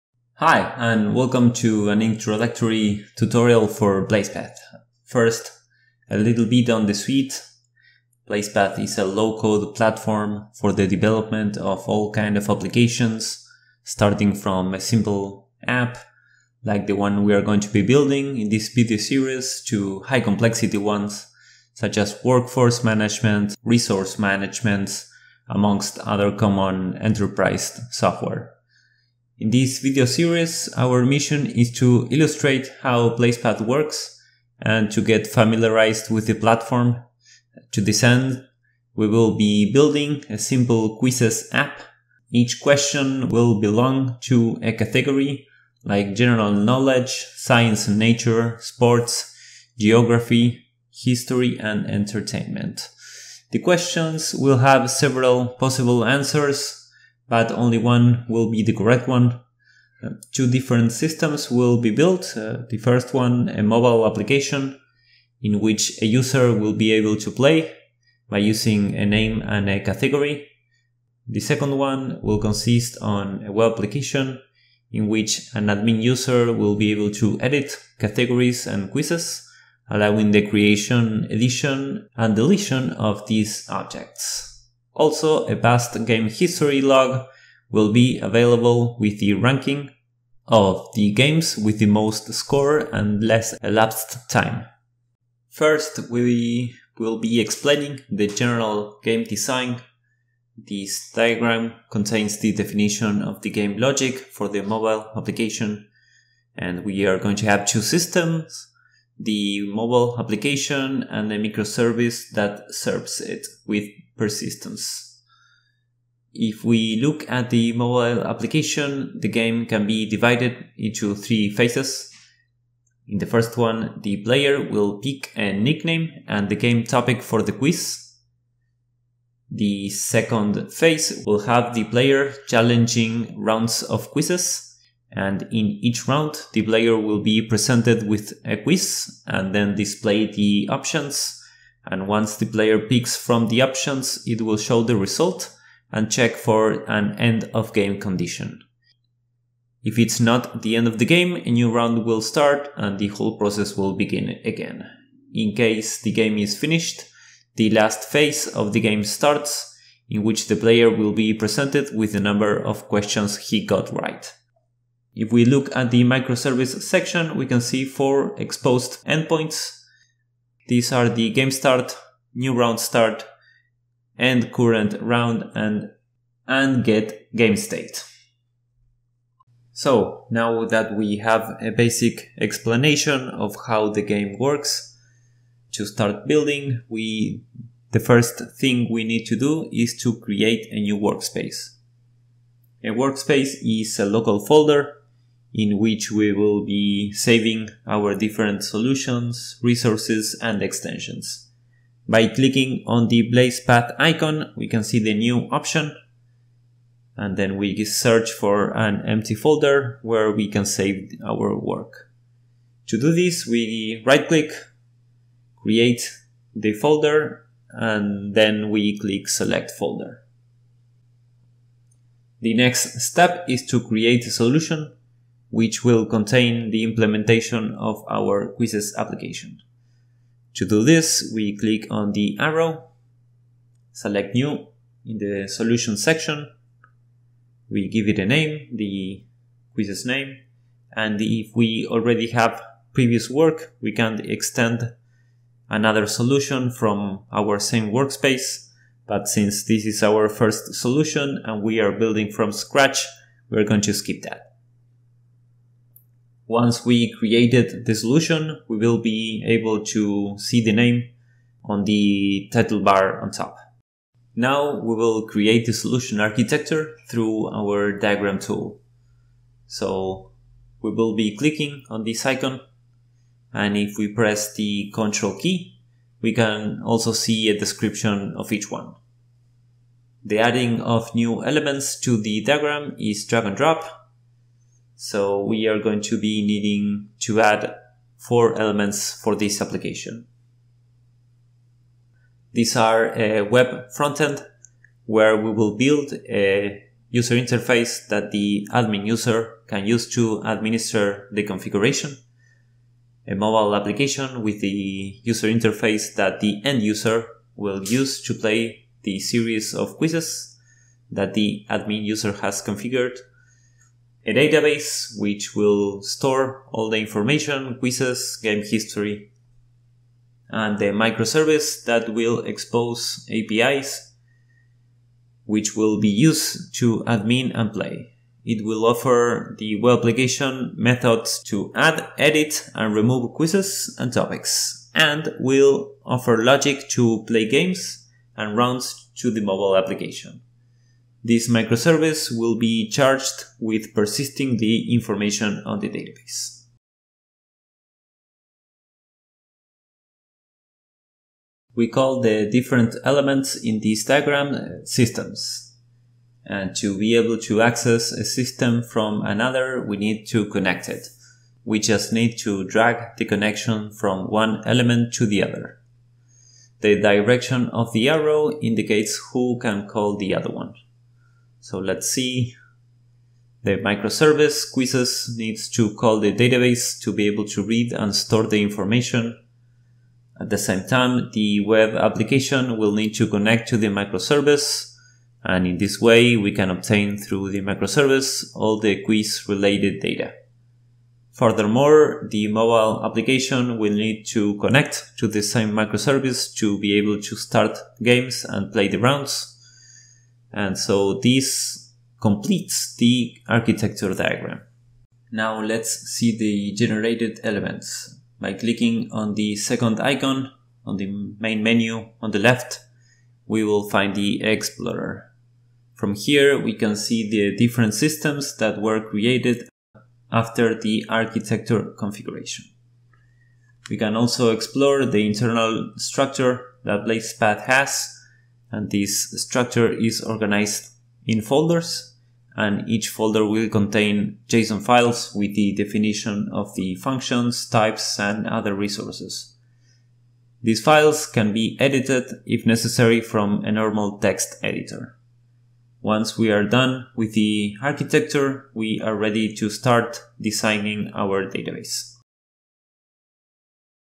01 | Blazedpath - Getting started
In this video we are going to touch on:
- Overview of the development of the project tutorial.

This is an introductory video series for getting started with blazedpath. We are going to Build an App from scratch!

Follow along the explanation and the tutorials to build a simple quizzes app, with web and mobile capabilities, all within the platform.

Blazedpath
Big Ideas Come Alive